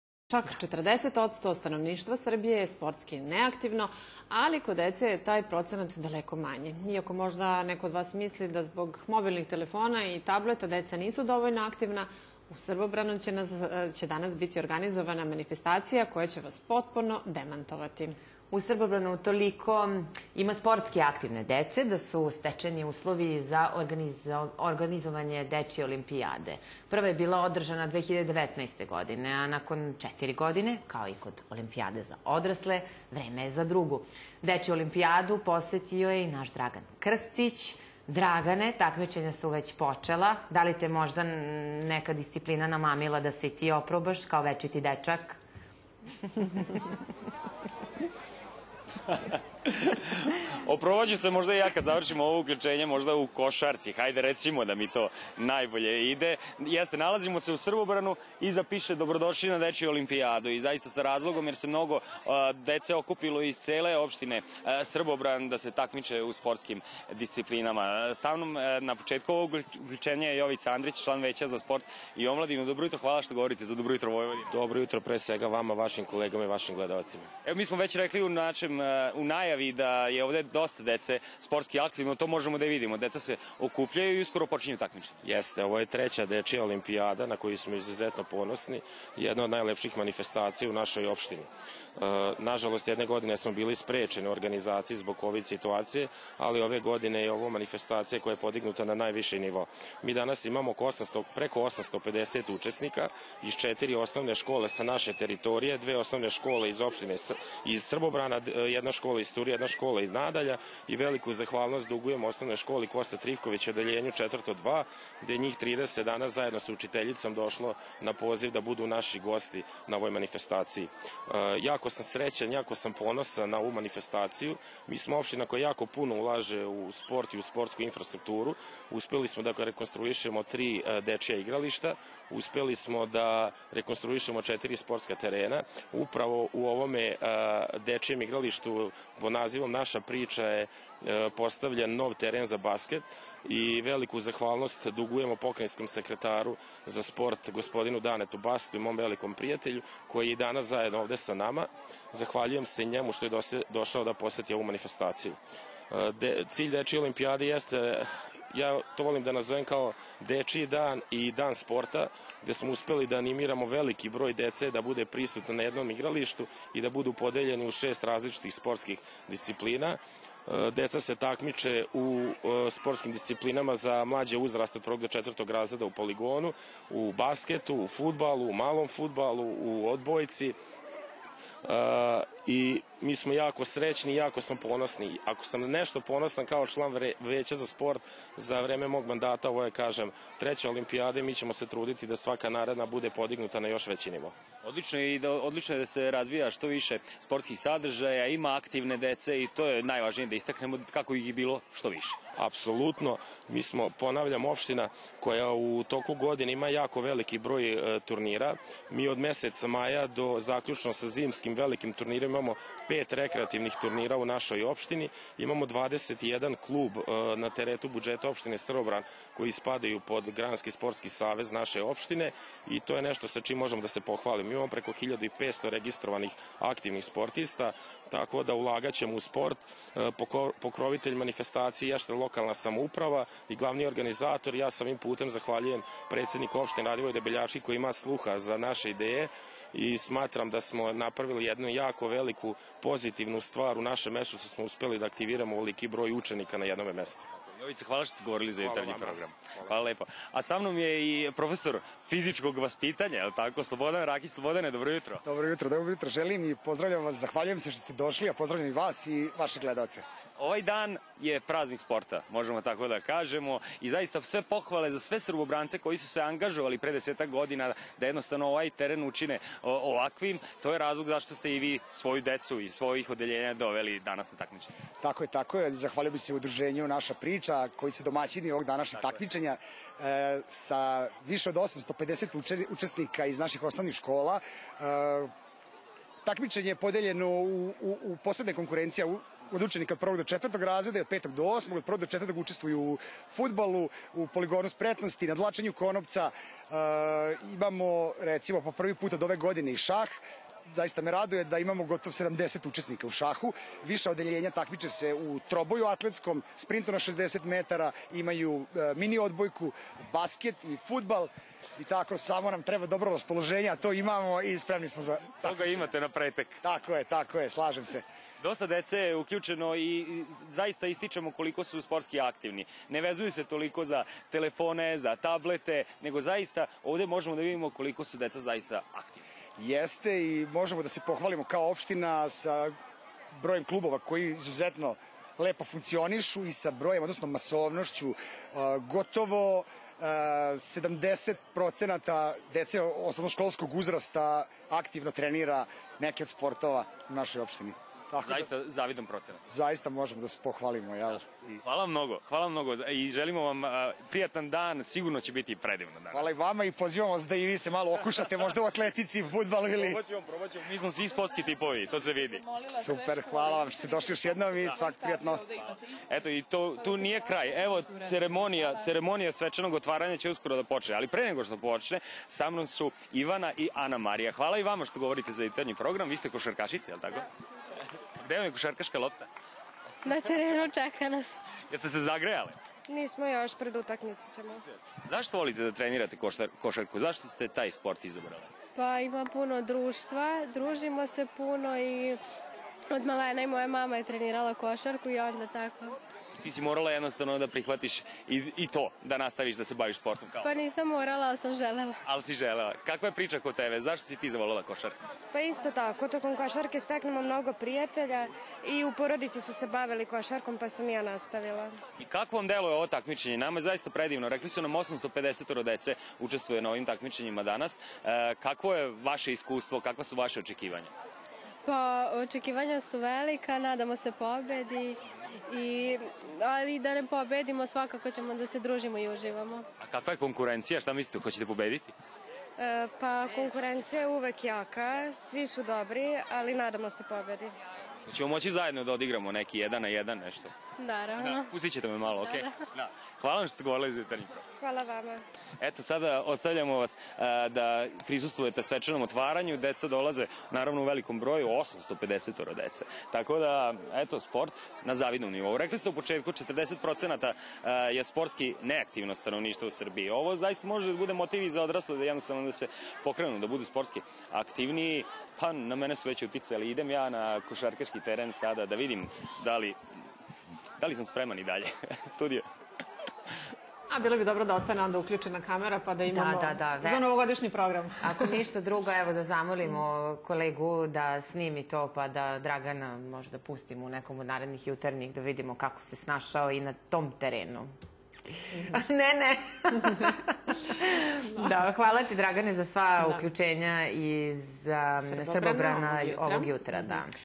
Dečija olimpiijada u Srbobranu
Čak 40 odsto stanovništva Srbije je sportski neaktivno, ali kod dece je taj procenat daleko manji. Iako možda neko od vas misli da zbog mobilnih telefona i tableta deca nisu dovoljno aktivna, u Srbobranu će danas biti organizovana manifestacija koja će vas potpuno demantovati. U Srbobranu toliko ima sportski aktivne dece da su stečeni uslovi za organizovanje Dečje olimpijade. Prva je bila održana 2019 godine, a nakon 4 godine, kao i kod Olimpijade za odrasle, vreme je za drugu.